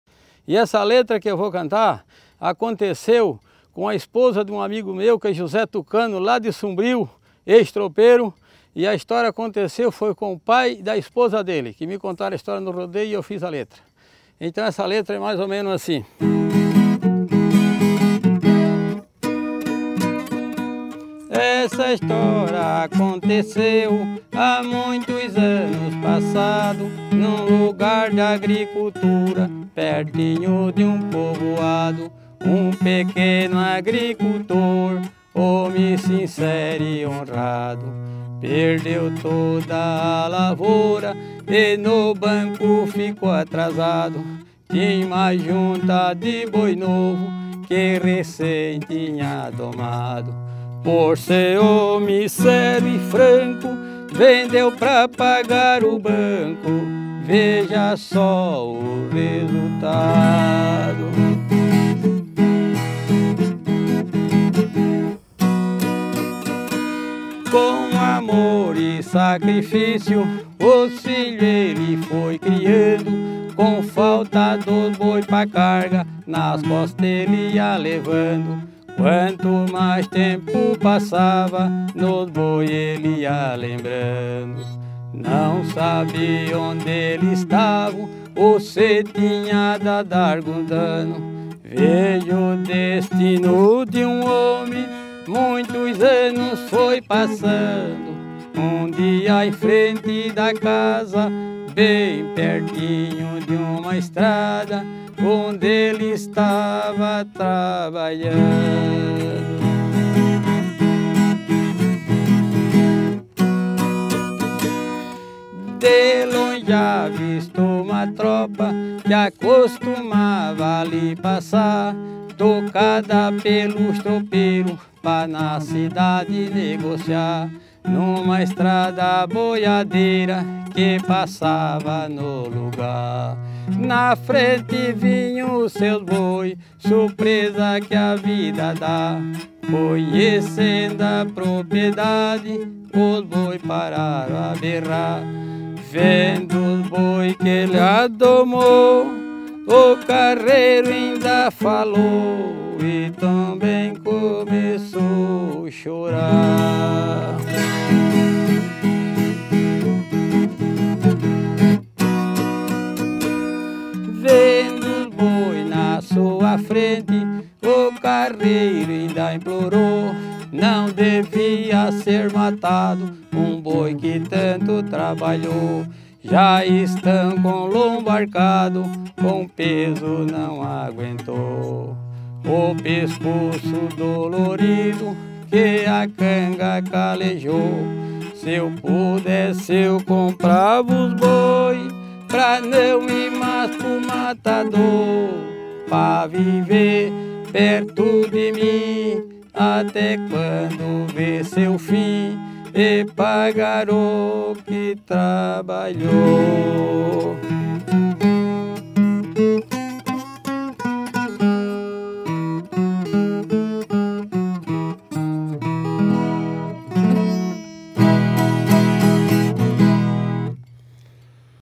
MÚSICA: O EMOCIONANTE REENCONTRO DOS BOIS E SEU CARREIRO!
Vocês pediram e a gente compartilha a música completa da história de um carreiro que reencontrou seus bois depois de muitos anos! Composição do agricultor e poeta Adelsom Firmino Cruz, morador de Jaguaruna (SC).